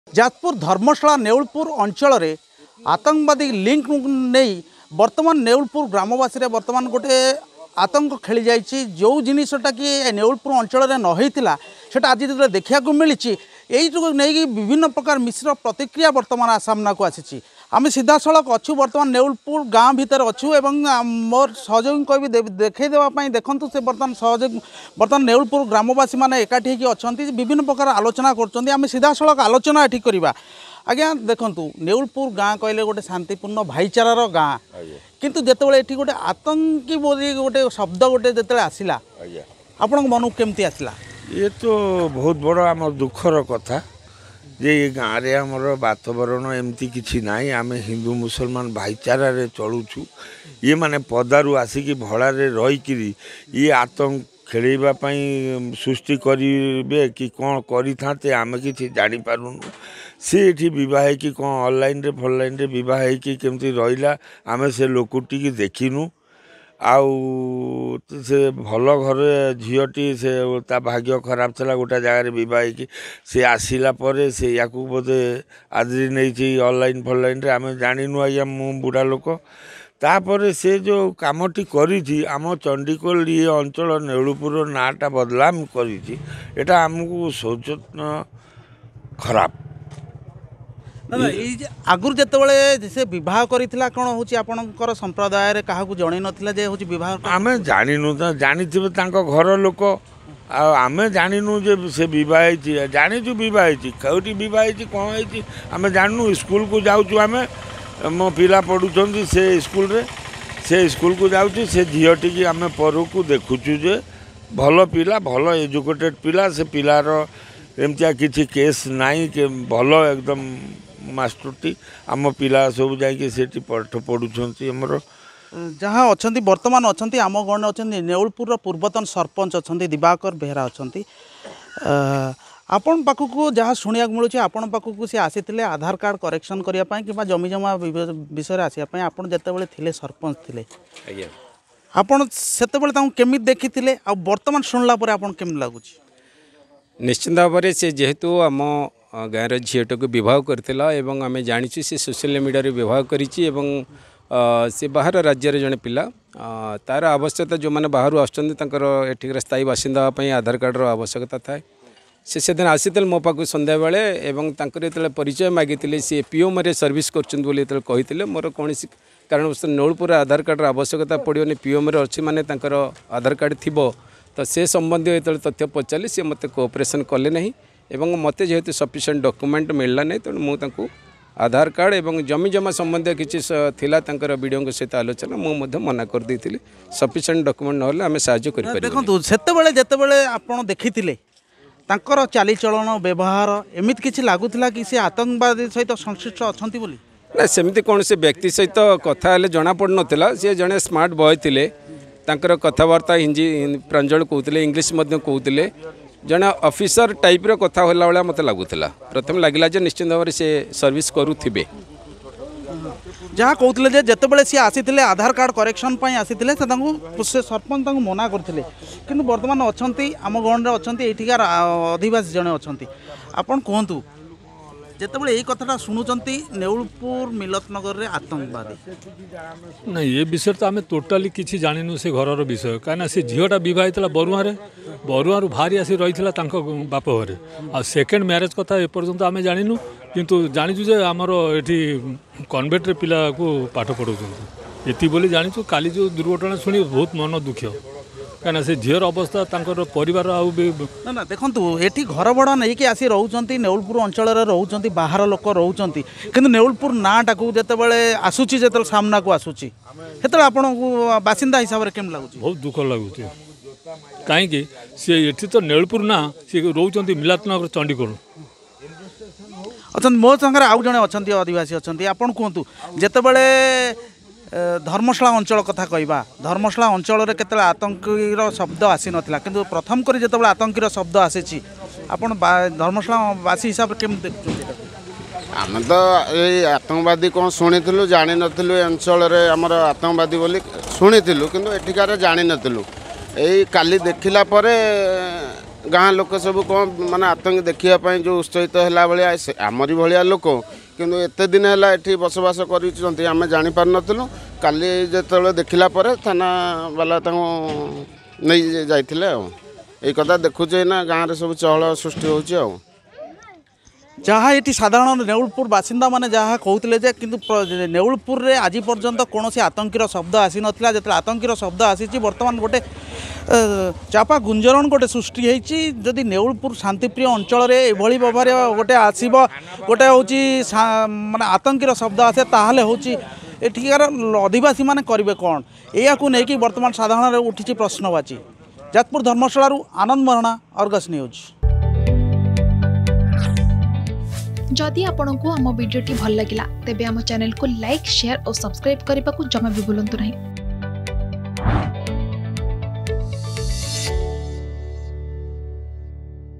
ନେଉଳପୁର ମିଲତ ନଗରରେ ଆତଙ୍କବାଦୀ || Jajpur Dharmasala
ନେଉଳପୁର ମିଲତ ନଗରରେ ଆତଙ୍କବାଦୀ । ଏହି ଖବର ଜଣାପଡିବା ପରେ କ'ଣ ରହିଛି ନେଉଳପୁର ଅଞ୍ଚଳ ଲୋକଙ୍କ ପ୍ରତିକ୍ରିୟା ଆସନ୍ତୁ ଜାଣିବା ।